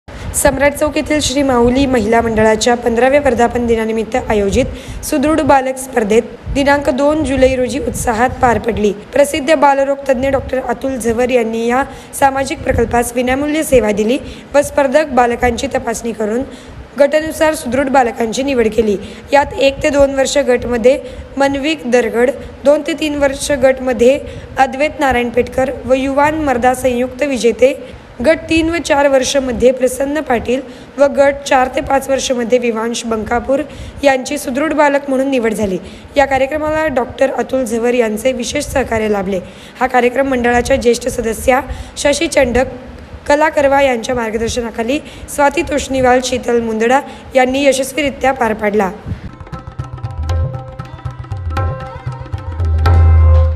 माऊली महिला मंडळाच्या वतीने सदृढ बालक स्पर्धा...
सोलापूर : सम्राट चौक येथील श्री माऊली महिला मंडळाच्या १५ व्या वर्धापन दिना निमित्त आयोजित सुदृढ बालक स्पर्धा दि. २ जुले रोजी उत्साहात पार पडली. प्रसिद्ध बालरोग तज्ञ डॉ.अतुल झंवर यांनी या सामाजिक प्रकल्पास विनामूल्य सेवा दिली व स्पर्धक बालकांची तपासणी करून गटनुसार सुद्ढ बालकांची निवड केली.
यात 1 ते 2 वर्ष गट मध्ये मनविक दरगड, 2 ते 3 वर्ष गट मध्ये अद्वेत नारायणपेठकर व युवान मर्दा संयुक्त विजेते, गट 3 ते 4 वर्ष मध्ये प्रसन्न पाटील व गट4 ते 5 वर्ष मध्ये विवांश बंकापूर यांची सुदृढ बालक म्हणून निवड झाली. या कार्यक्रमाला डॉ.अतुल झंवर यांचे विशेष सहकार्य लाभले. हा कार्यक्रम मंडळाच्या ज्येष्ठ सदस्या शशी चंडक, कला करवा यांच्या मार्गदर्शनाखाली सौ.स्वाती तोष्णीवाल व सौ. शीतल मुंदडा यांनी यशस्वीरीत्या पार पडला.